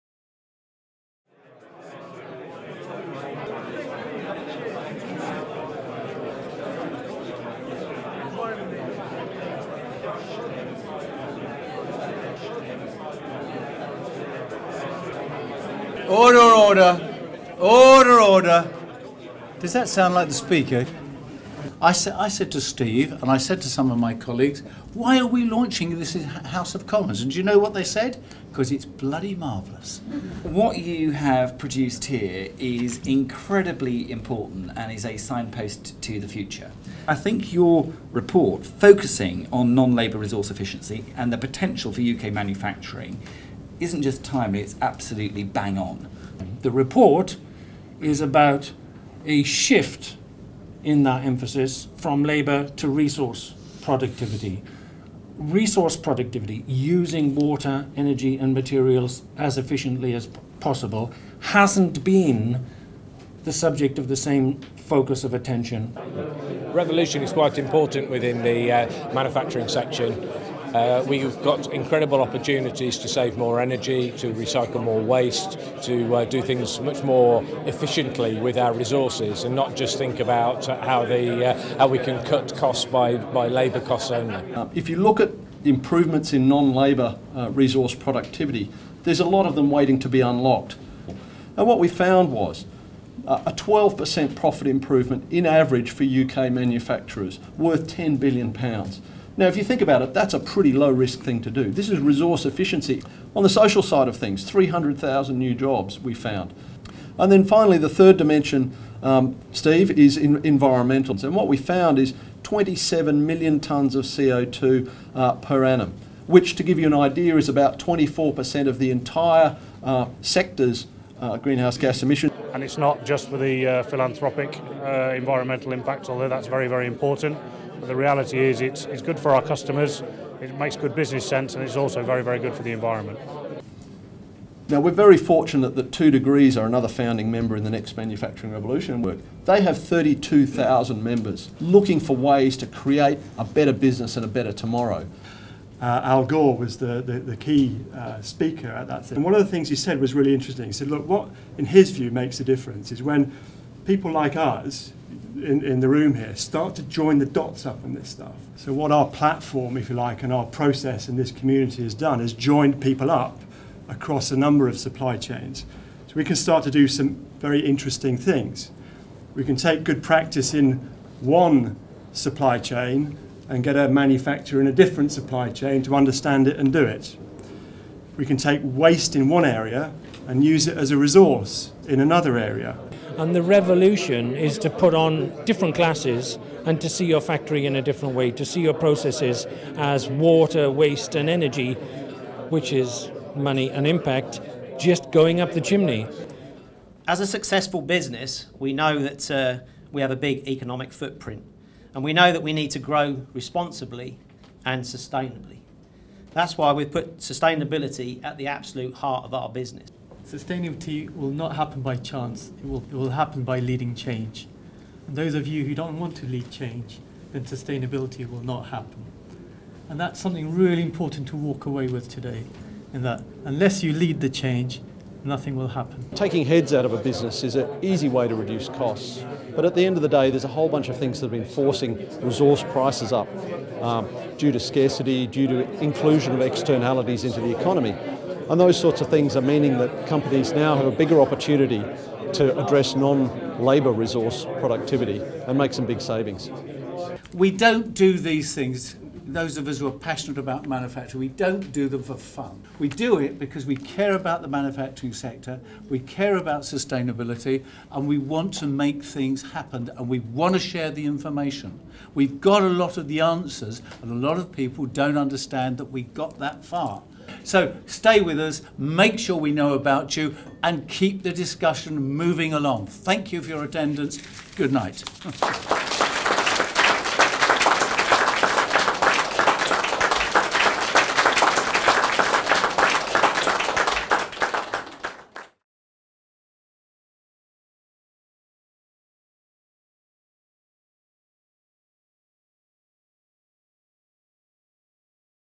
Launch of the Next Manufacturing Revolution
The Next Manufacturing Revolution (NMR) has launched its landmark report and program to help UK manufacturers improve their non-labour resource productivity. The report identifies the significant untapped opportunities for UK manufacturing in non-labour resource efficiency and outlines a programme of action to address the barriers to uptake.